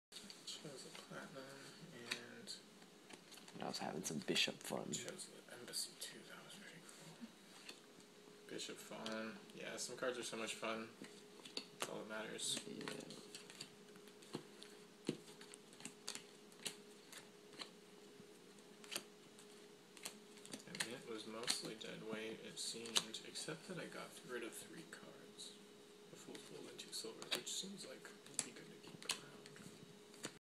Hamster in hoodie
Sogeking chills in his papa's hoodie